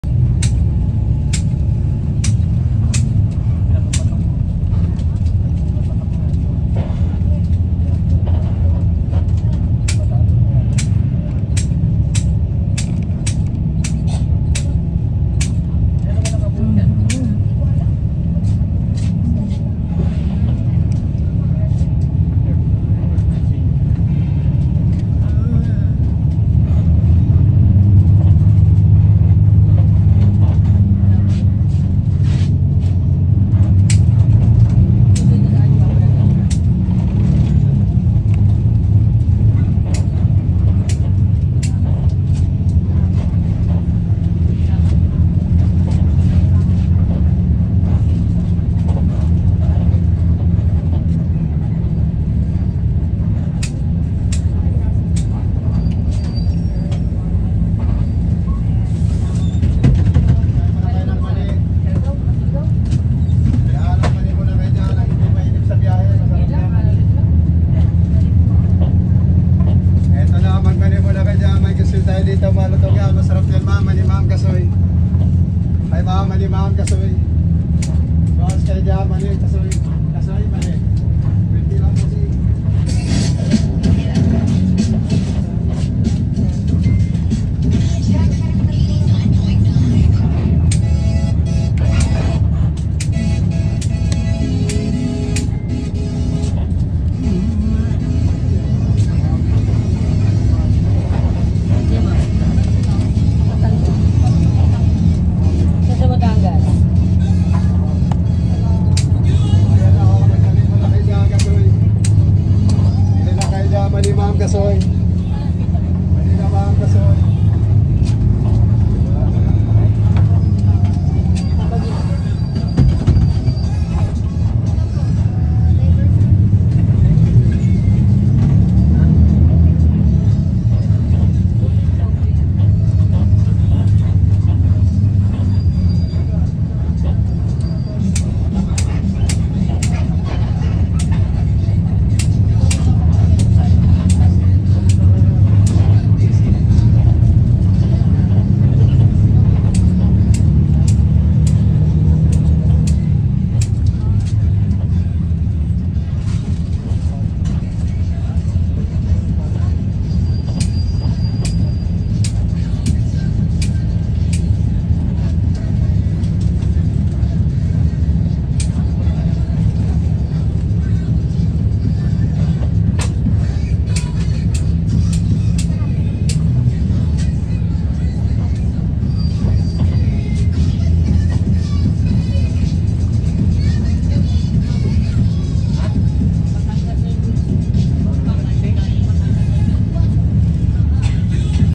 PITX to BATANGAS CITY JAM Bus Trip Window View Pt 1
Rainy afternoon bus journey on the way to Batangas City
Sept 2024